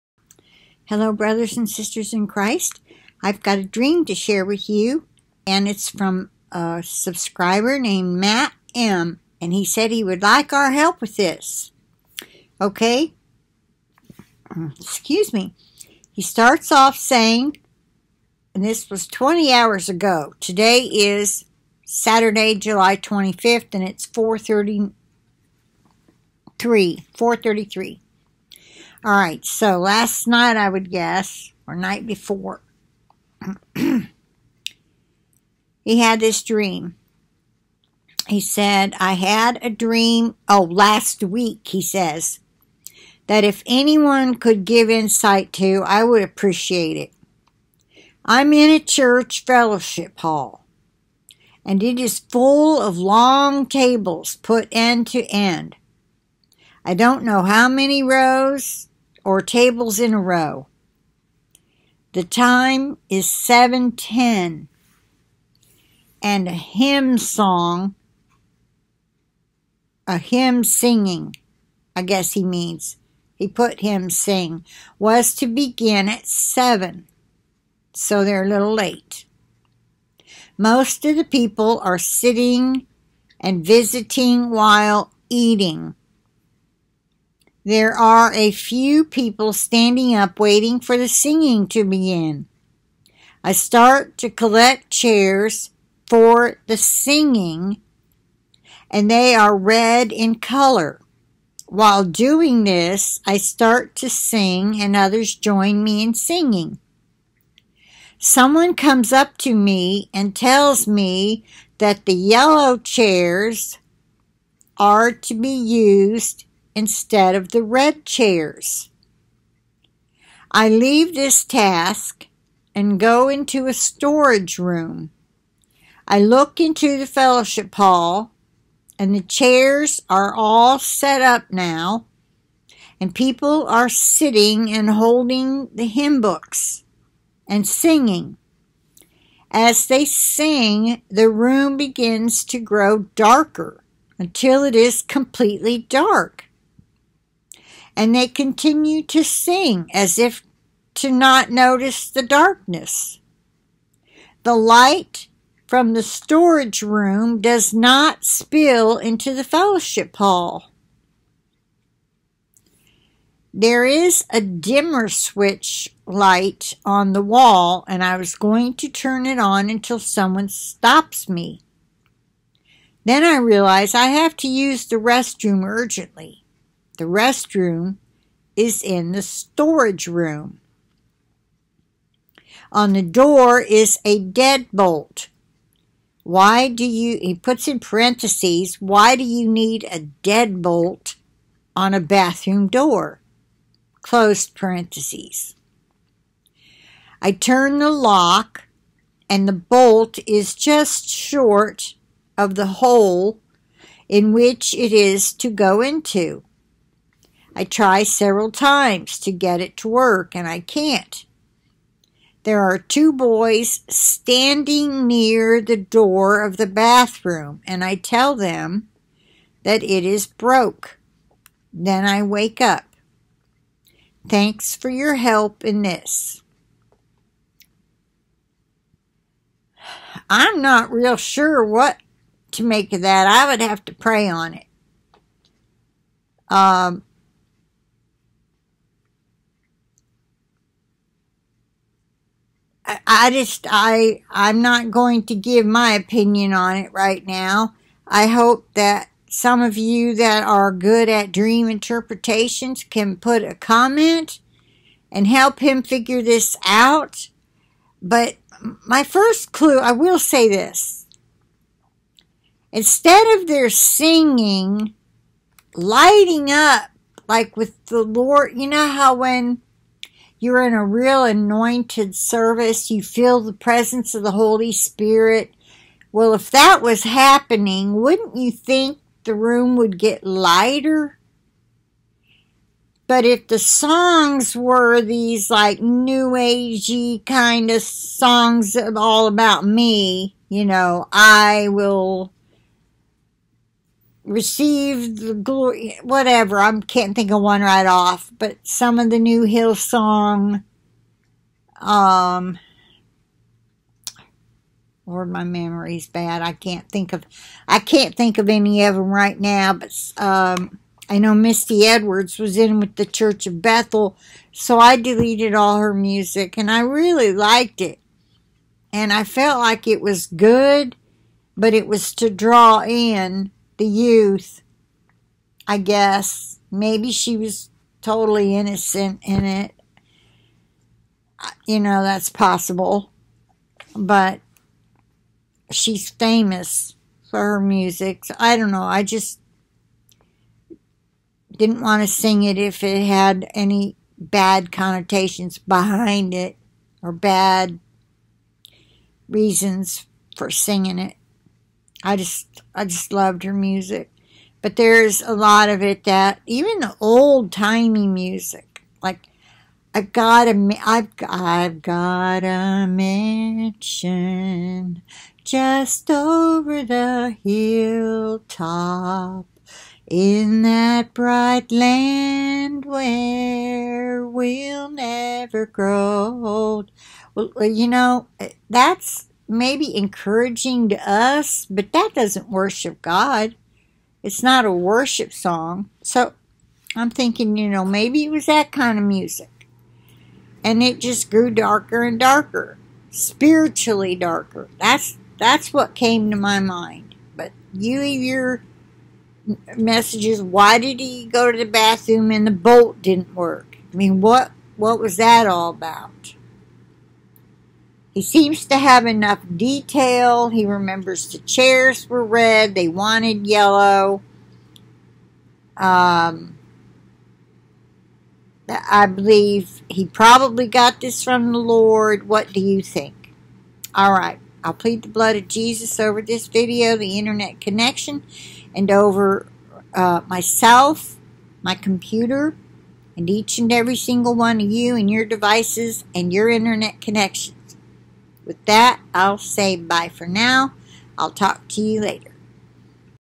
CAN YOU HELP FIGURE OUT THIS DREAM???
This dream is about a church singing, and the singing was late, and caused the light in the room to get darker and darker, until there was no light at all.